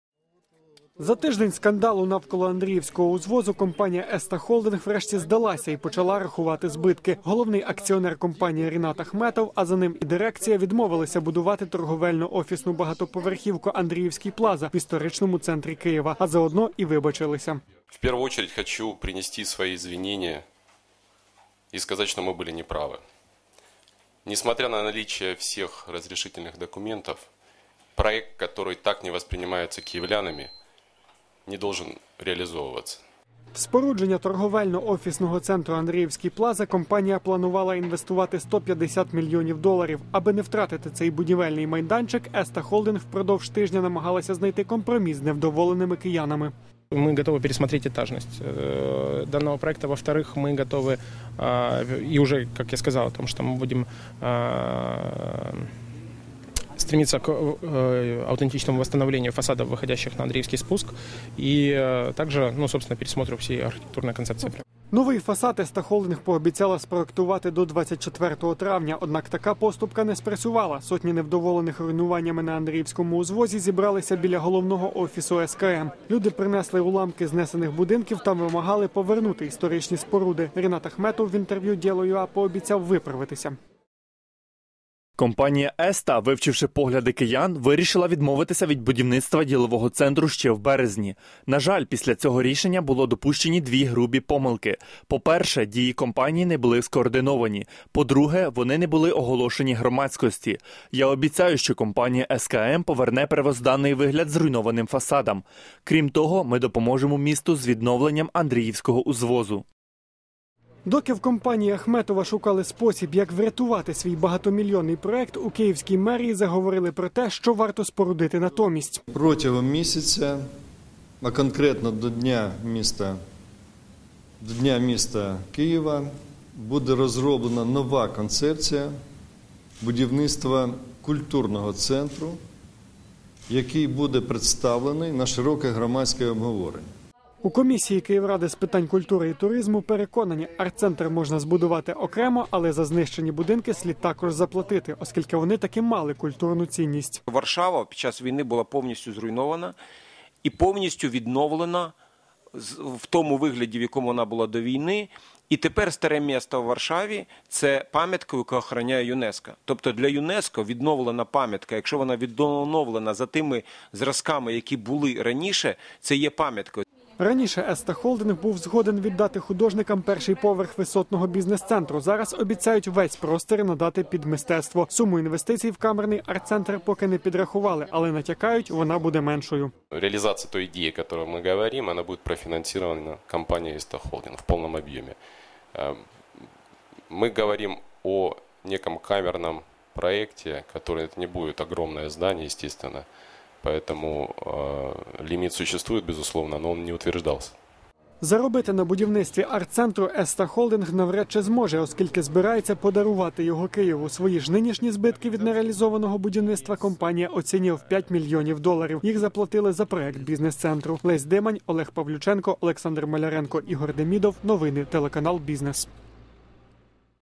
Ахметов відмовився від бізнес-центру на Андріївському
Після гучного скандалу навколо Андріївського узвозу, компанії "Еста Холдинг" Ріната Ахметова все ж довелося відмовитися від нового бізнес-центру в історичному центрі Києва. Група СКМ зазнала мільйонних збитків, "плюс" до того змушена відновити фасади зруйнованих будинків, побудувати там мистецький центр і подарувати його столиці.